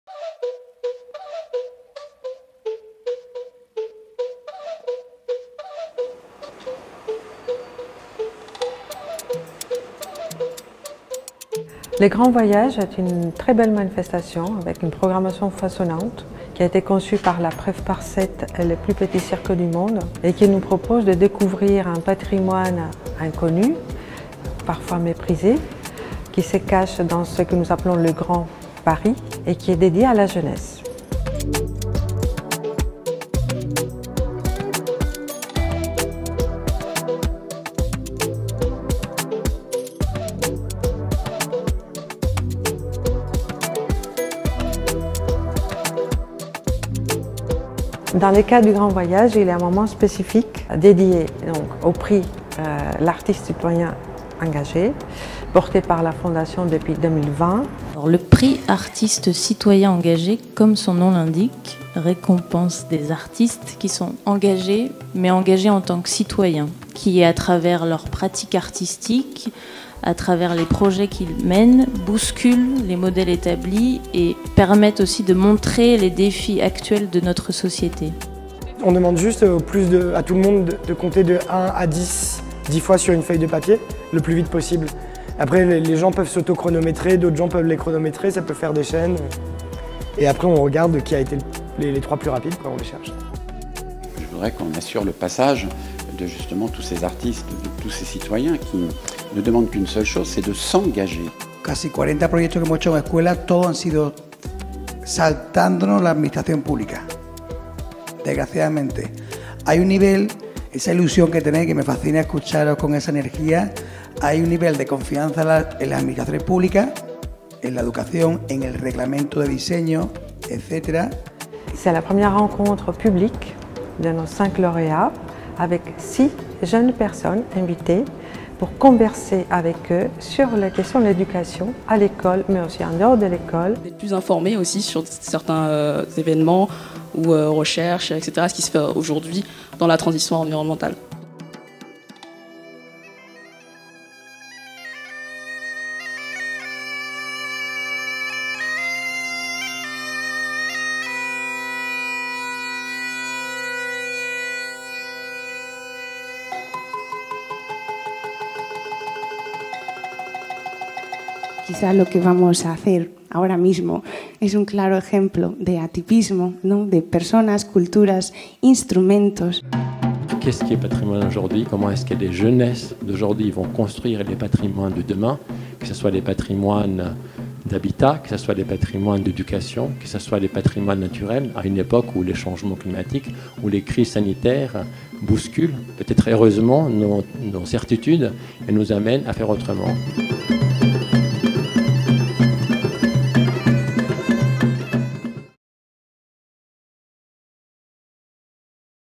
1ère rencontre des lauréats 2020 du Prix Artiste citoyen engagé
Le Prix Artiste citoyen engagé, décerné par la Fondation, récompense des artistes qui s’engagent, en France comme en Espagne, en tant que citoyens pour transformer les modèles établis avec des actions remarquables face aux défis sociétaux. La première édition du Prix (2020) compte 5 artistes primés : deux artistes français, Neïl Beloufa et Patrick Bouchain, et trois artistes espagnols, Santiago Cirugeda, Julio Jara et Cristina Pato. À travers leurs parcours et leurs actions, ces artistes se sont associés à des aventures collectives aussi originales qu’exemplaires autour d’enjeux citoyens et solidaires. Chaque lauréat bénéficie d’une dotation financière (30 000 €) et d’une valorisation de son action.

El Premio Artista comprometido reconoce la labor de artistas que se comprometen como ciudadanos y ciudadanas, tanto en Francia como en España, para transformar los modelos establecidos mediante acciones destacables en respuesta a diferentes problemáticas sociales. La primera edición del Premio (2020) cuenta con 5 artistas premiados: tres artistas españoles, Santiago Cirugeda, Julio Jara y Cristina Pato, y dos franceses, Neïl Beloufa y Patrick Bouchain. El Premio recalca el valor de prácticas artísticas exigentes, impulsadas por los artistas y desarrolladas en el marco de proyectos colectivos y sociales. Cada uno de los premiados recibe una dotación económica (30.000 €) y ve su labor reconocida y difundida.